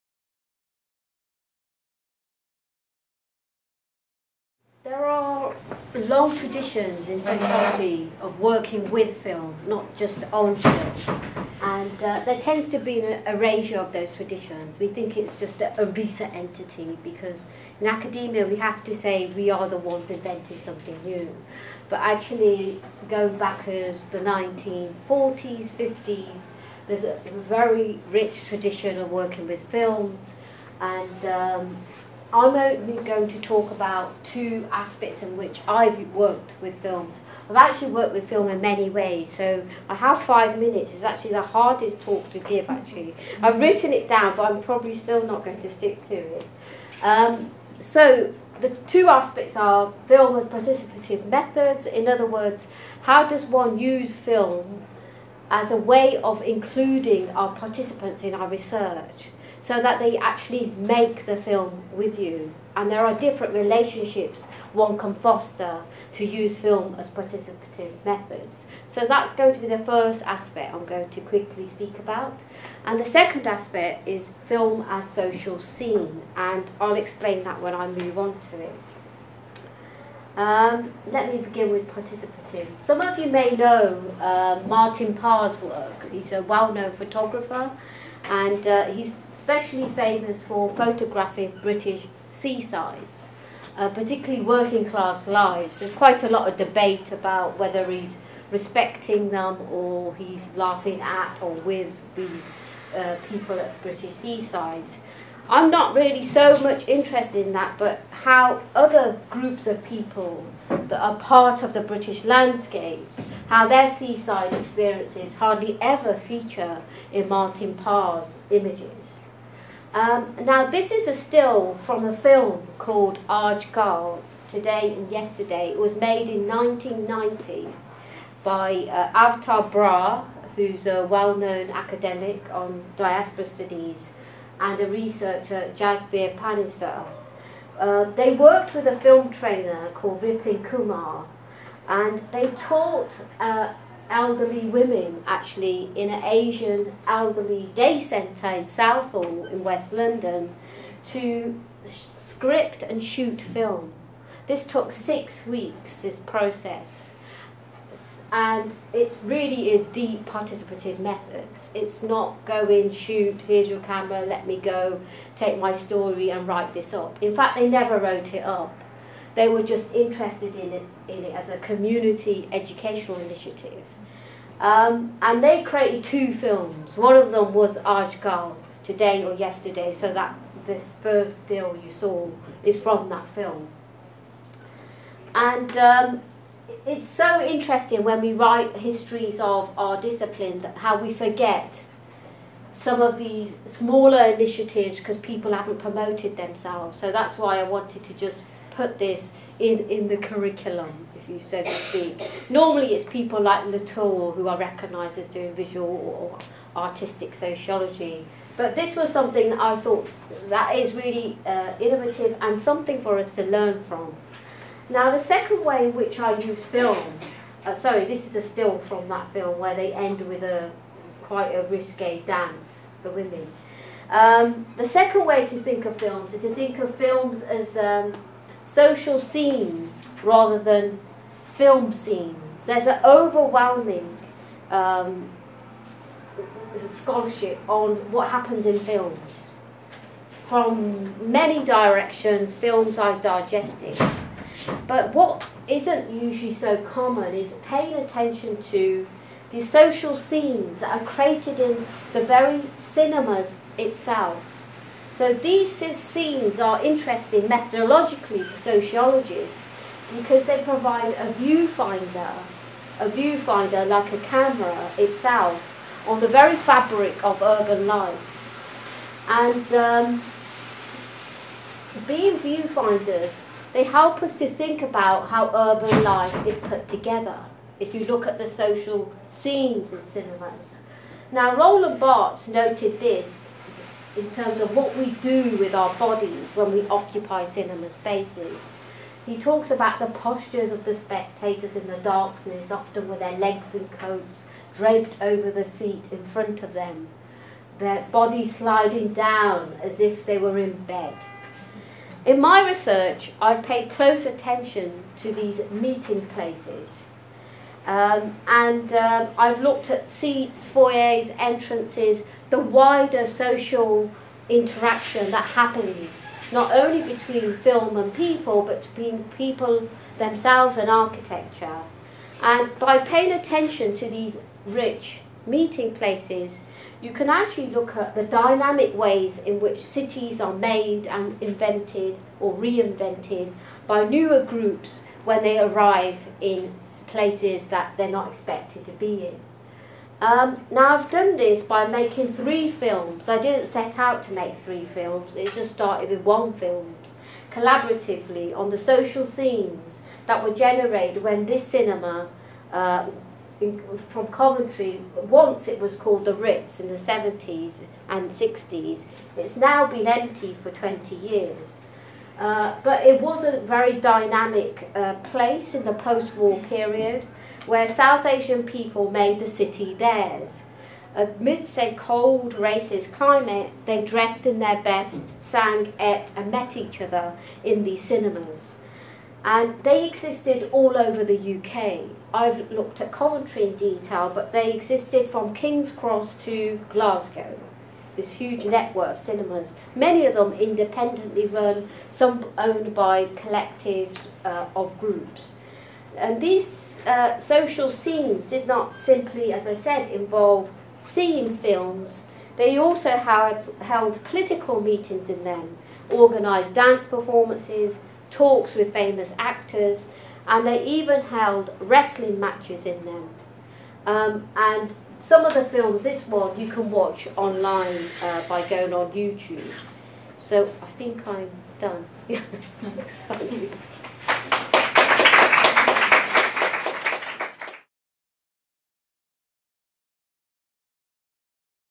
How to do Sociology with ... Movies | Dr Nirmal Puwar
At Goldsmiths, we practise Sociology with a variety of things, from buildings to music and social media. But how do we do this? Let us explain...